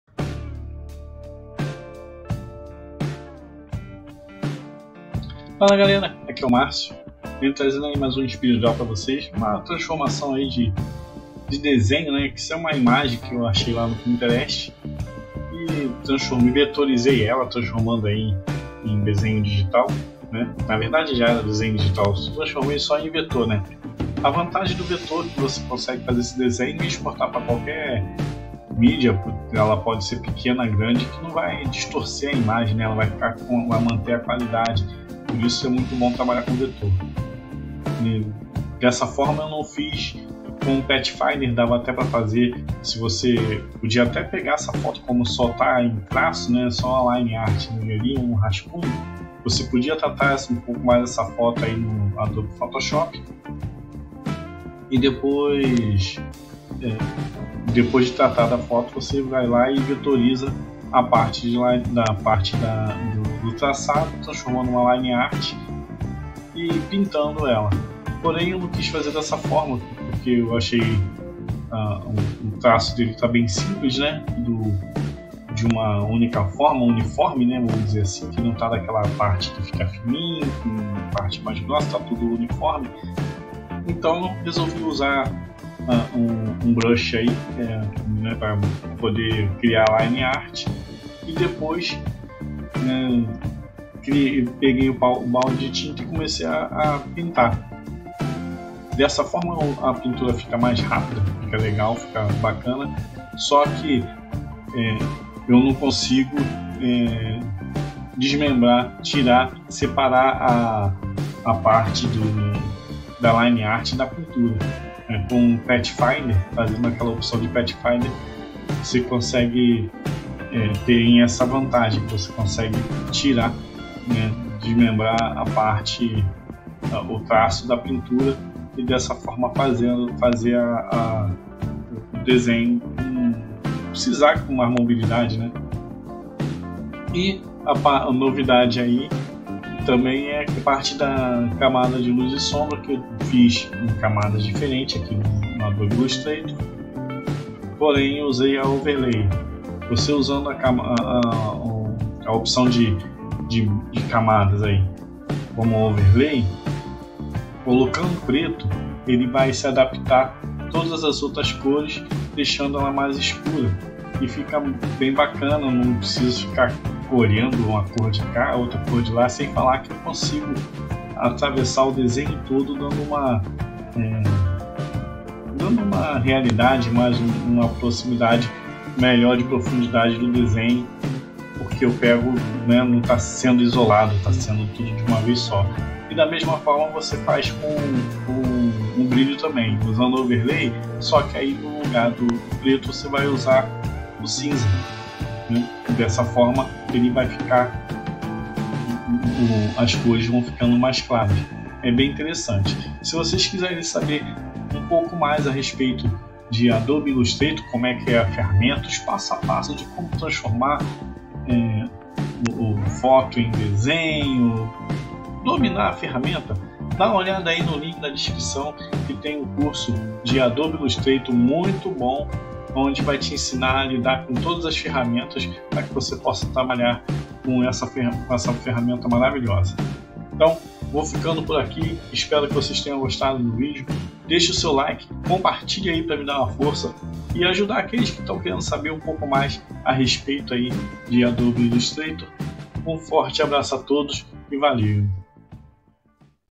Vetorizando Desenho
Aí está mas uma transformação de desenho em vetor.
A imagem base foi encontrada la no pinterest, como é apenas um esboço, ficou fácil para treinar vetorização.
Se você deseja aprender todas as ferramentas do adobe ilustrator.